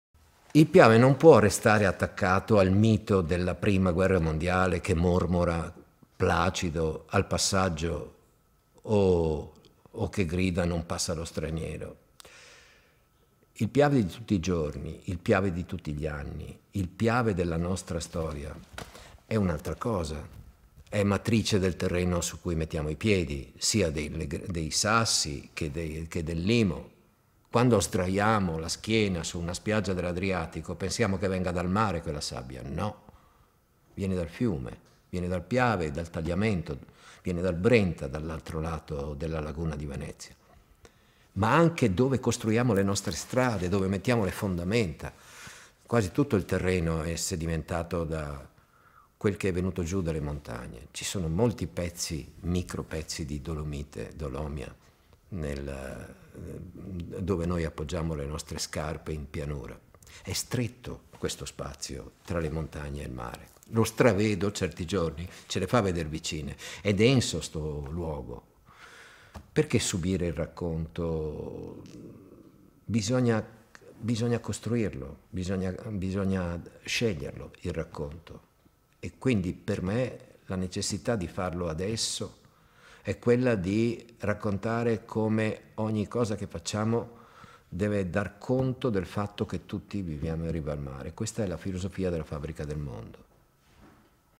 Venezia - ACQUA. RIECCO PAOLINI: TEATRO DI PREVENZIONE CIVILE DA MARMOLADA AL MARE -4- (18.01.24)
- Venezia - ACQUA. RIECCO PAOLINI: TEATRO DI PREVENZIONE CIVILE DA MARMOLADA AL MARE
Venezia, 16 lug. - Dopo VajontS 23, Marco Paolini torna a fare teatro di 'prevenzione civile'. Il nuovo progetto si chiama 'Mar de Molada', è sostenuto da Regione Veneto e Veneto Agricoltura: sarà un racconto corale a tappe sui prati del bacino del Piave, dalla Marmolada fino a Venezia. Per quattro fine settimana da metà settembre all'inizio di ottobre, dall'alba al tramonto, chiamerà a raccolta per un racconto del territorio partendo non dalla terra ma dalle acque: le fonti, i torrenti, i fiumi le valle, la laguna, elementi fondamentali della vita, del modello di sviluppo, dell'agricoltura, delle città. Si partirà dai Serrai di Sottoguda, ai piedi della Marmolada. Lì sei anni fa, un piccolo torrente, il Pettorina, affluente del Cordevole, a sua volta affluente del Piave, ha trascinato via tutto quello che caratterizzava il meraviglioso paesaggio di quella gola. Tra poche settimane quella gola, dopo il restauro ad opera di Veneto Acque in collaborazione con la Regione, tornerà visitabile. 'Mar de Molada' partirà, dice Paolini, "proprio da questo simbolo della fragilità del luogo in cui viviamo e scenderà con appuntamenti settimanali lungo il Piave per arrivare sull'Adriatico, a Vallevecchia, l'ultimo pezzo di costa veneta agricola dove ancora si legge un paesaggio diverso da quello del turismo. È stretto lo spazio tra le montagne e il mare: lo stravedo certi giorni, ce le fa vedere vicine, è un luogo denso. Oggi siamo tutti preoccupati di essere vittime di un territorio fragile. Mi chiedo: perché subirne il racconto? Bisogna costruirlo, bisogna essere abitanti e sceglierlo". Il racconto vuole "dar conto del fatto che tutti viviamo in riva al mare. Questa è la filosofia del progetto Fabbrica del Mondo". Il primo appuntamento di teatro campestre sarà all'alba di sabato 14 settembre sui prati di Malga Ciapela (Belluno) per Mar de Molada Serrai di Sottoguda, il 21 settembre alle 15 a Sospirolo (Treviso) dove il Cordevole sta per buttarsi nel Piave ci sarà Mar de Molada Certosa di Vedana, la settimana successiva ancora lungo il Piave per Mar de Molada Grave del Montello, per chiudere il 5 ottobre alle 16.30 con Mar de Molada Vallevecchia nella Brussa, poco sopra l'estuario del Piave. Per creare questo progetto Paolini è partito con lo studio della documentazione dell'Arpav e delle fonti regionali per quel che riguarda l'assetto idrogeologico, si è confrontato con scienziati e tecnici esperti che nelle Università e negli enti regionali si occupano di acqua. 'Mar de Molada' sarà un lavoro corale. Sul palco assieme a Paolini ci saranno Patrizia Laquidara, poliedrica artista, tra le voci più intense e liriche della musica d'autore contemporanea, che accompagnerà il racconto con il canto, Giovanni Frison, musicista e compositore delle arie dello spettacolo e un coro di narratori popolari. E poi ad ogni appuntamento cori, bande, e conversazioni con scienziati e tecnici che racconteranno alcuni aspetti della vita del Piave e degli altri fiumi veneti. "Ogni volta ci sarà lo spettacolo, sempre un po' diverso, ma anche conversazioni. Non venite col tempo risicato, venite per prendervi una giornata per camminare o andare in bicicletta, per stare sulle rive, per prendere in mano un sasso, per guardare quello che ci sta intorno. Perché lo spettacolo non sarà solo sul palcoscenico, sarà essere noi, 2.000 persone sedute sul prato, che senza lasciar dietro di sé nemmeno una carta per terra si porteranno via qualcosa che dura e che ci lega ai fiumi della nostra terra", dice Paolini. Gli spettacoli si svolgeranno con modalità rispettose dell'ambiente ospitante e ogni volta con ospiti diversi. L'afflusso del pubblico sarà regolato da un biglietto d'accesso 'posto prato unico' in vendita su Vivaticket (12 euro più commissioni). "Dalla Marmolada a Venezia, dalle montagne al mare: un viaggio alla scoperta di un territorio fragile, in cui l'acqua scandisce e determina lo sviluppo sostenibile, sociale, ma anche economico del Veneto. Una regione che ha compreso l'importanza della risorsa idrica e ha avviato una gestione sostenibile per affrontare sfide come la crescita della popolazione, il cambiamento climatico e la contaminazione- commenta il presidente della Regione, Luca Zaia- 'Mar de Molada' è un evento che nasce non solo per esplorare luoghi inediti, ma anche per fare prevenzione nel vero senso del termine. Un'attività non più procrastinabile, soprattutto considerato il fenomeno del cambiamento climatico, mirata a evitare o a ridurre i possibili danni in caso di calamità. (18.01.24)